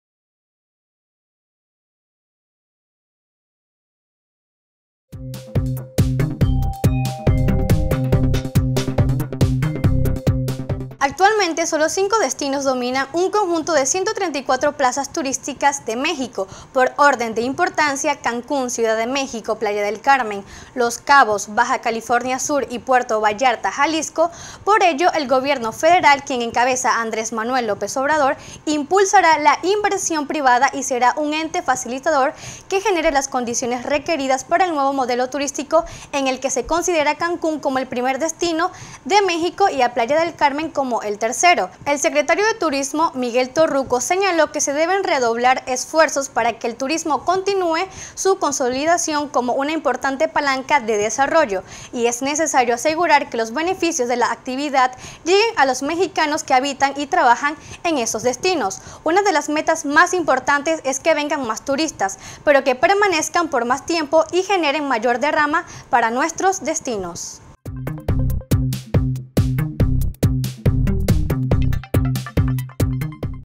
CANCÚN, PRIMER DESTINO DE MÉXICO; PLAYA DEL CARMEN, EL TERCERO
Entérate de las noticias “EN CORTO”…CANCÚN, PRIMER DESTINO DE MÉXICO; PLAYA DEL CARMEN, EL TERCERO...